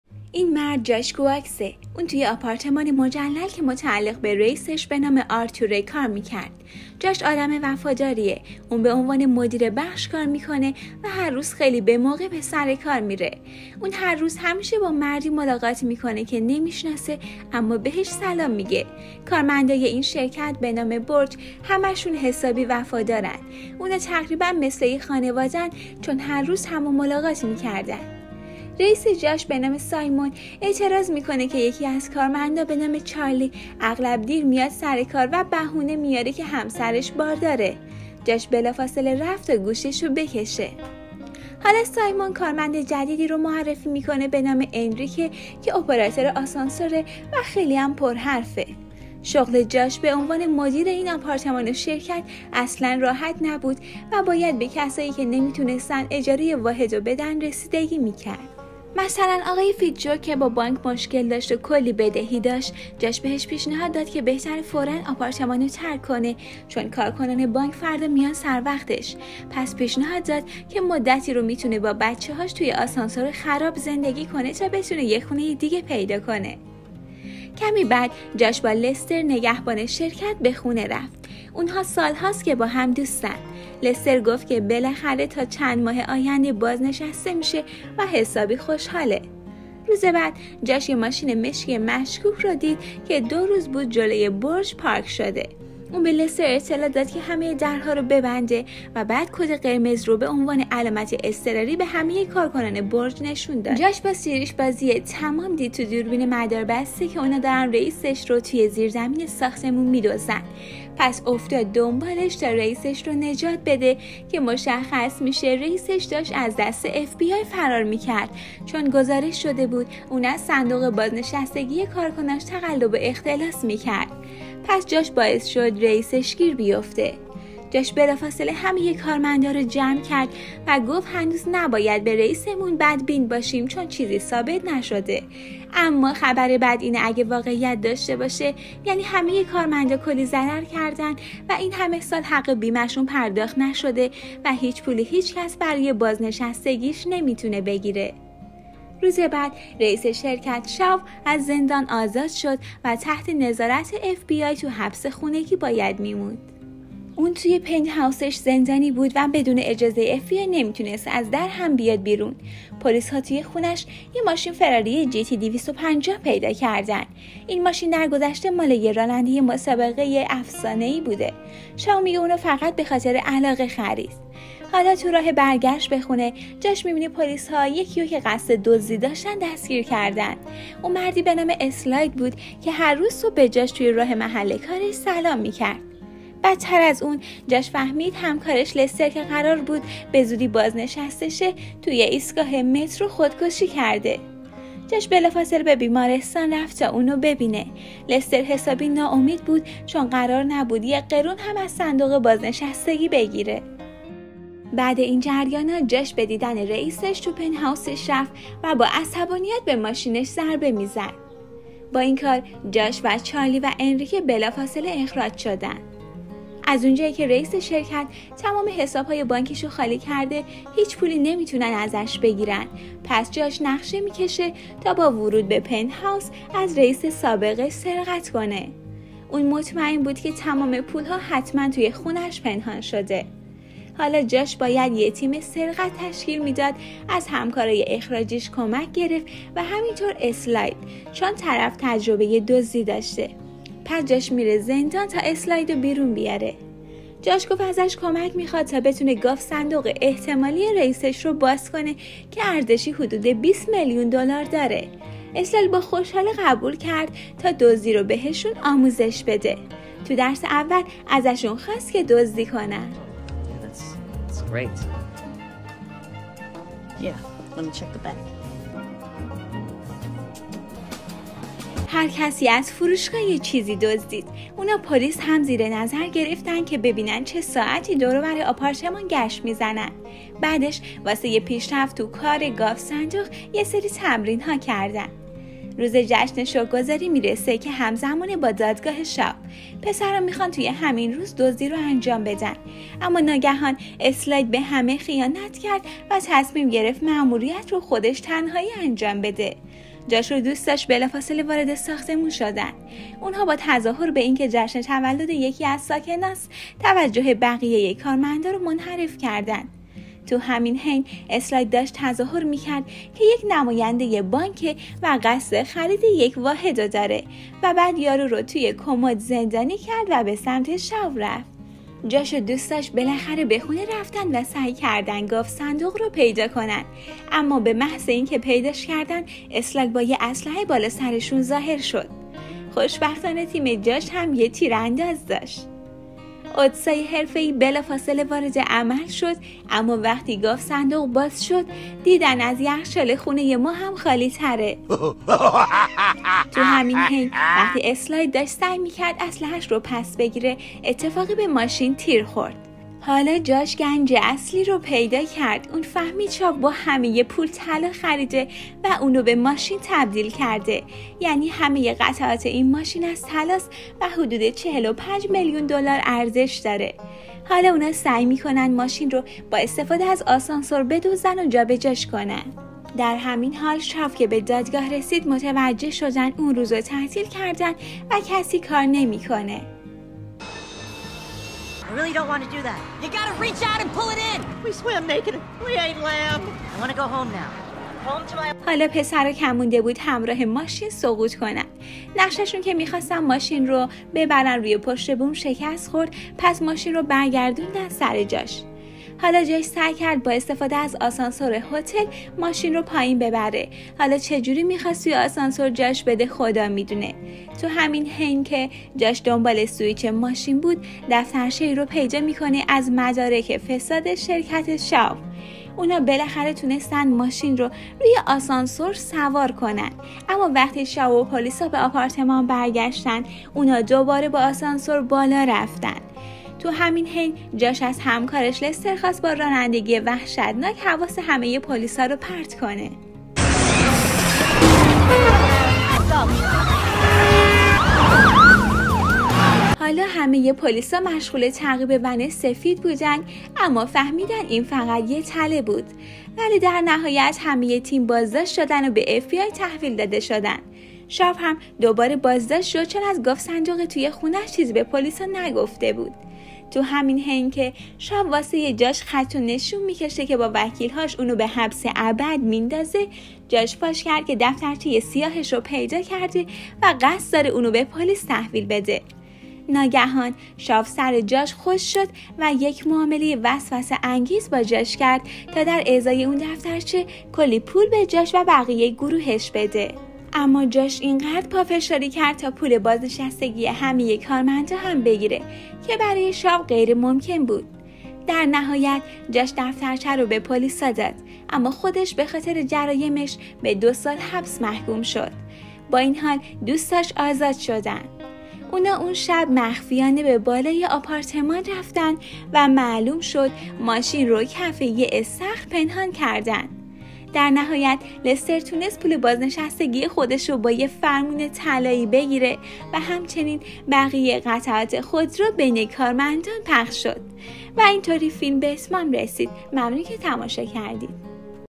تلاش برای سرقت یه ماشین فراری که از طلا ساخته شده😍❤️فیلم دوبله فارسی بدون سانسور
داستان تلاش های جالب برای سرقت یه ماشین. تمام قعطاتش هم از طلاست
ممنون میشم با لایک و سابسکرایب حمایت کنید❤️